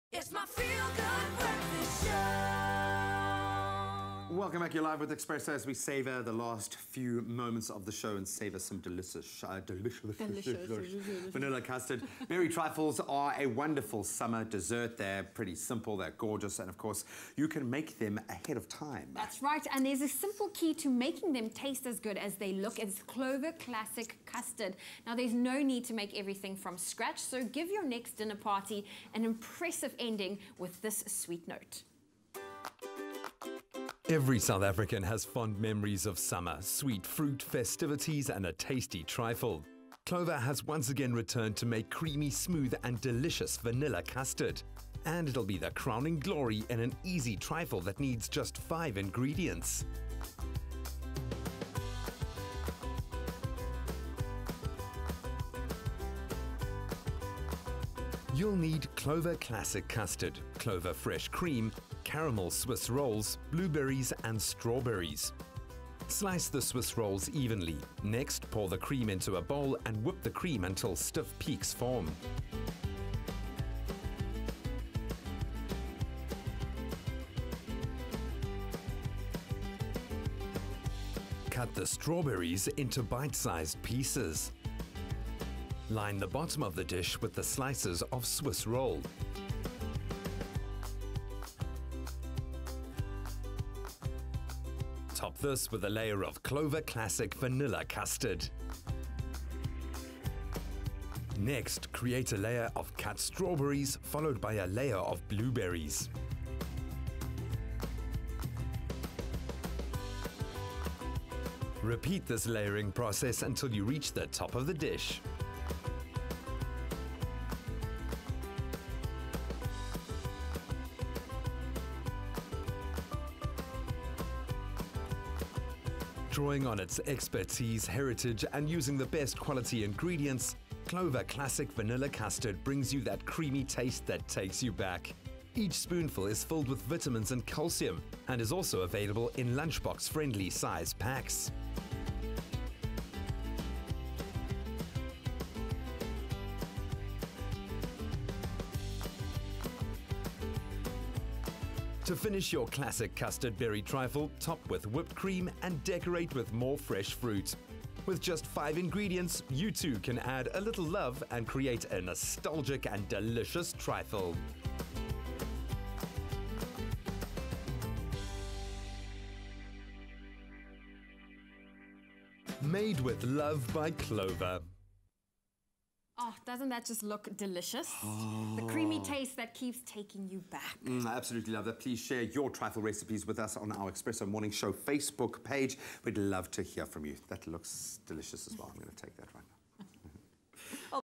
Clover 5 Ingredient Fix: Classic Custard Berry Trifle (CLOVER)
Berry trifles are wonderful summer desserts — they’re simple, gorgeous and you can make them ahead of time. But here’s the key to making them taste as good as they look: Clover Classic Custard!